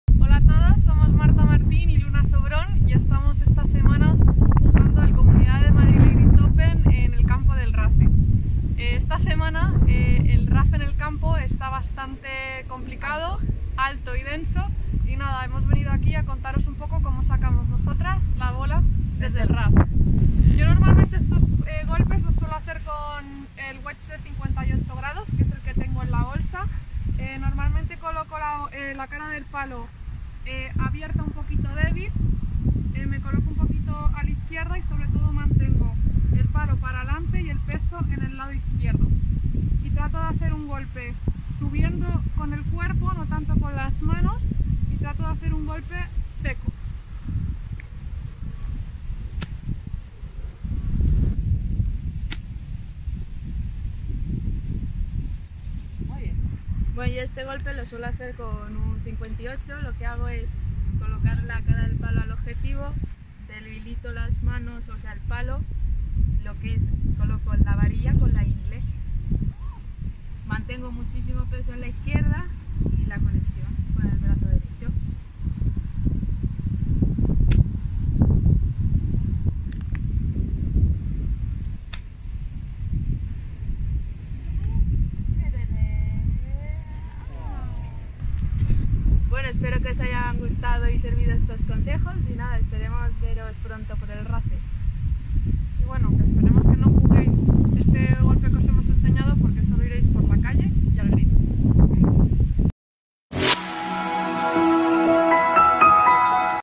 Luna Sobrón y Marta Martín nos enseñan a jugar desde el rough denso
En la previa del Comunidad de Madrid Ladies Open, las jugadoras de la Federación de Madrid nos muestran sus diferentes maneras de jugar un golpe alrededor de green con un rough muy espeso.